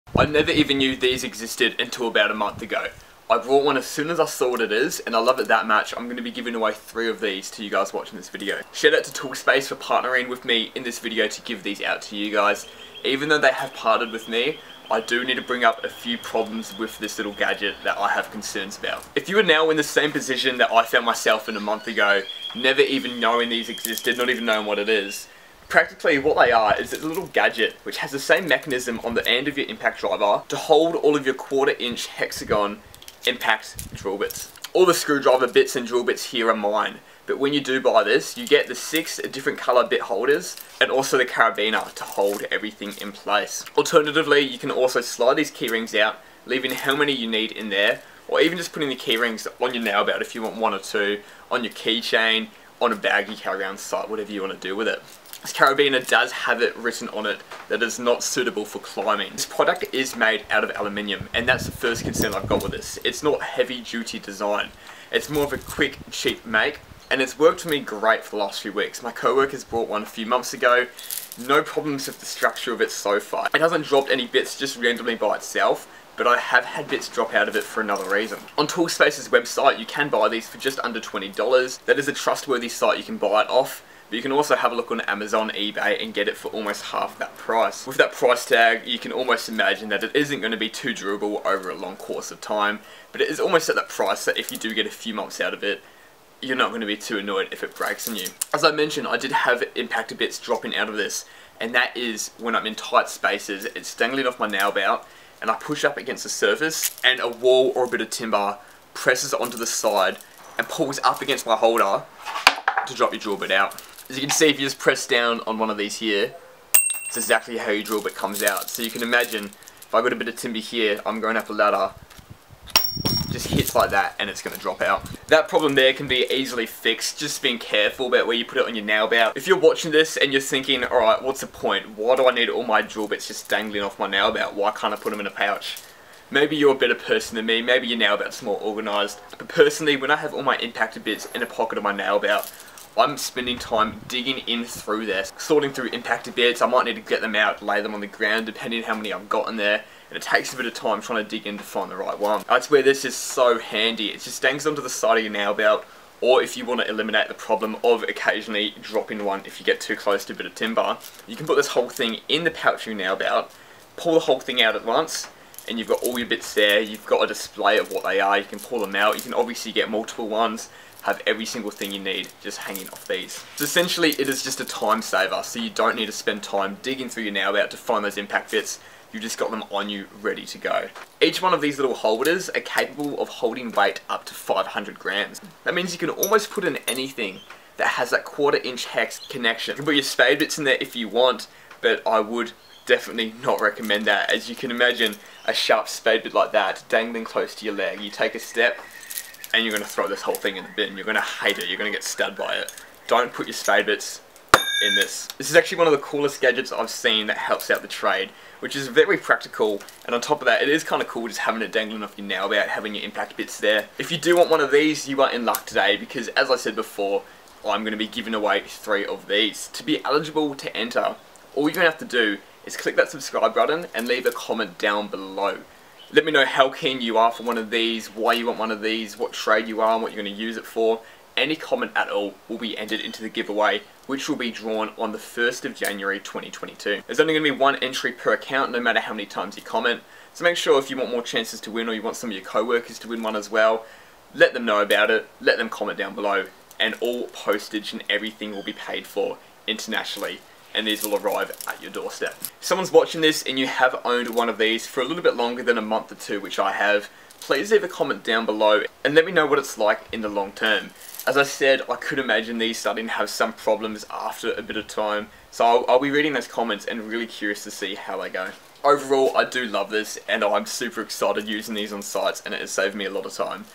Screw Driver Bit Holder Review
Information on the GIVE AWAY is in the middle of this video!
I only found out that these Hex shank bit holder key rings existed a few months ago. I made a TikTok of it straight away, and with 1.9million views it's easy to see I'm not the only person excited by this!
I used the bit holder on site for a few weeks so I can work out its flaws and capabilities and share them with you in this video!

This tool is super efficient for all trades when you are needing to switch between screw driver bits constantly and don't want to dig through your nail belt every time.